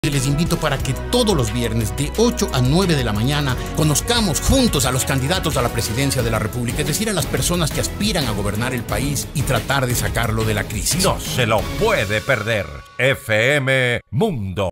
Decisión Ecuador 2021 con Jorge Ortiz
DecisiónEcuador2021 con JorgeOrtiz. Cada viernes desde las 08h00 conéctate a FMmundo 98.1. En una entrevista directa, frontal y sin tapujos, conoceremos a los candidatos presidenciales.

Te esperamos!